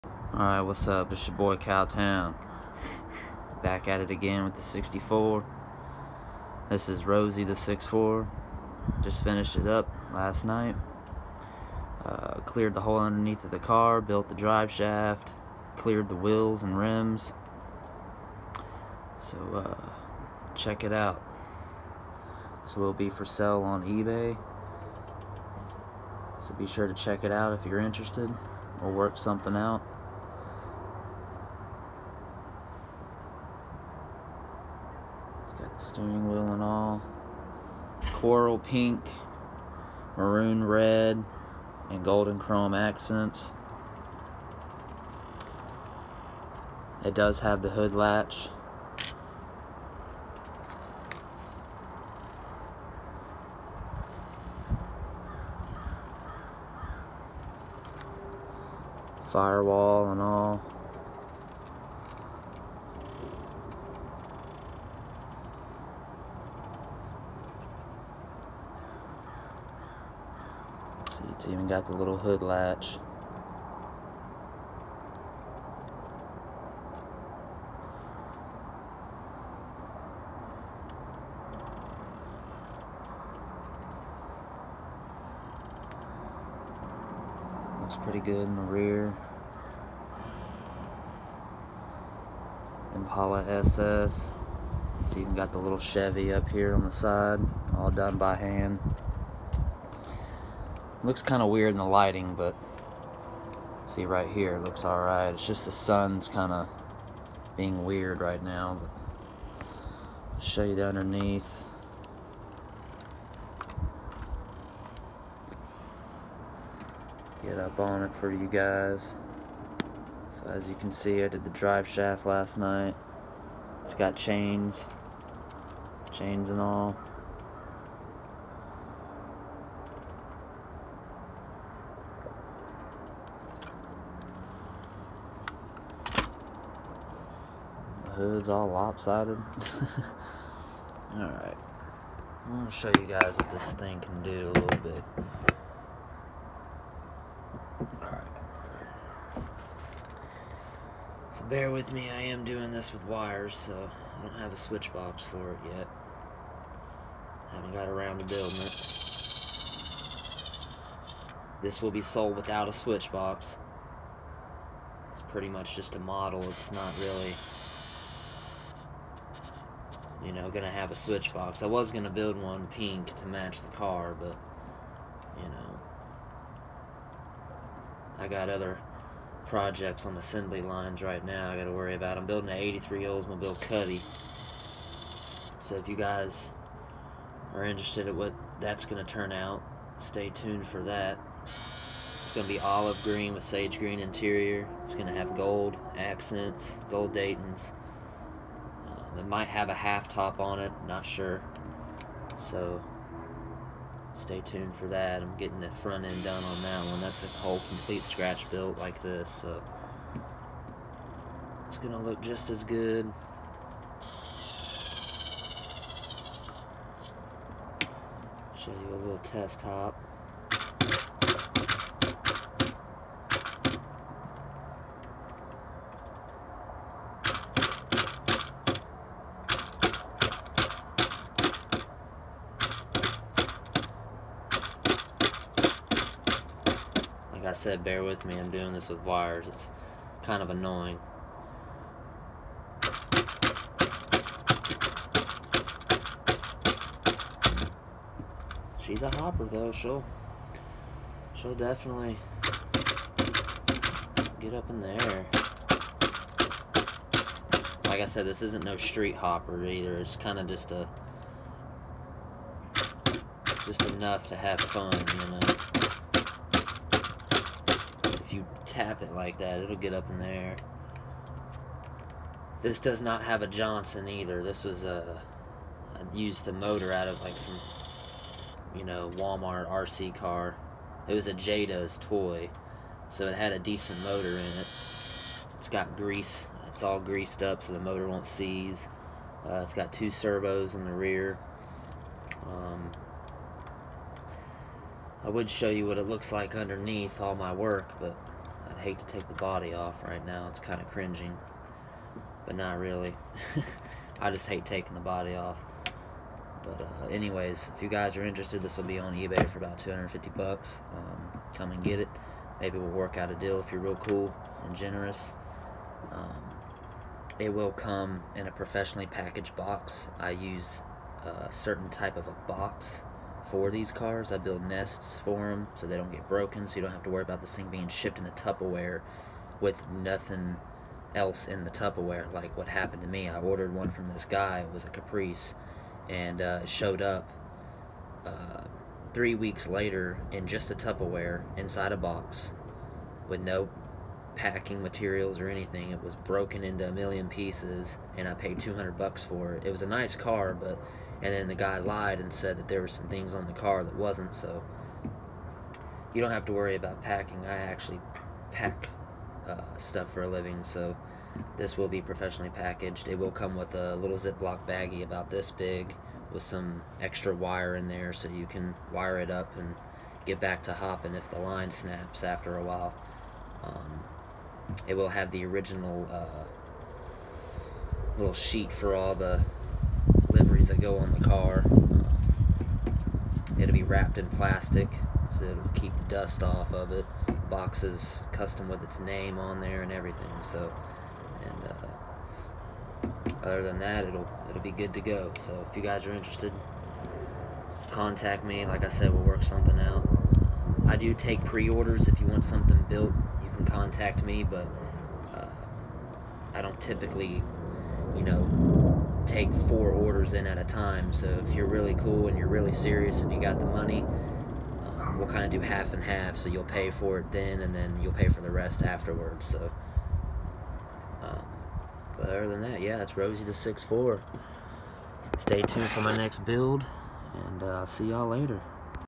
Model Lowrider Hopper '64
This is a custom build done by me. This car will be for sale check it out on eBay.